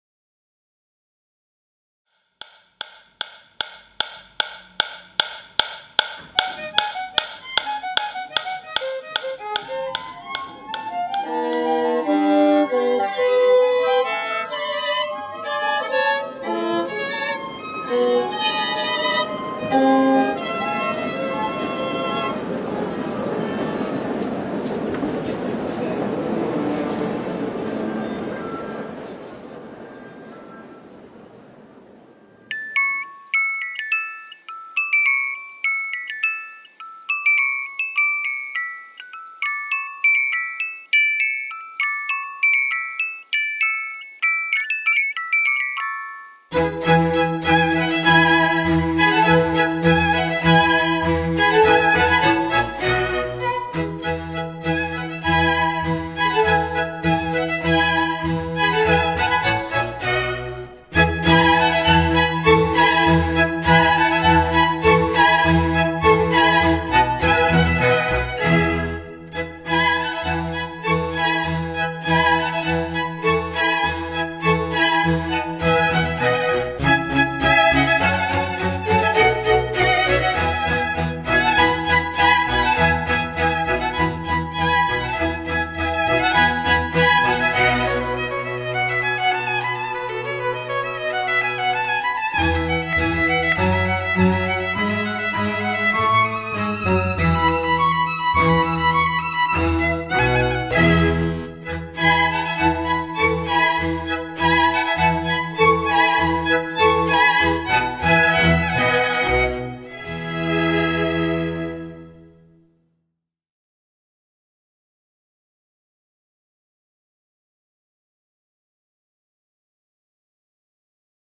Ulysse Nardin Stranger music box wristwatch plays Vivaldi
The Ulysse Nardin Stranger music box wristwatch plays Vivaldi. The watch is powered by the brand's in-house developed musical mechanical movement featuring silicon technology.